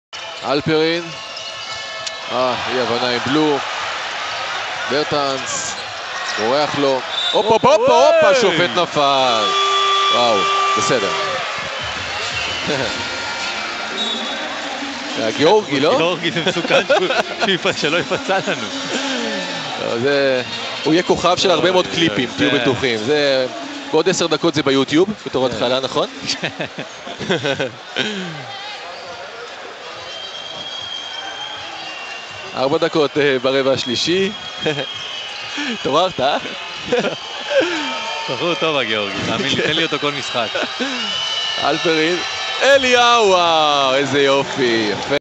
A comic moment during Israel vs Latvia
Just because Eli Sahar asked for that...

Fortunately the fall finished without any damage and with some laugh :)

Also a nice dunk by Lior Eliyahu, assist by Yotam Halperin.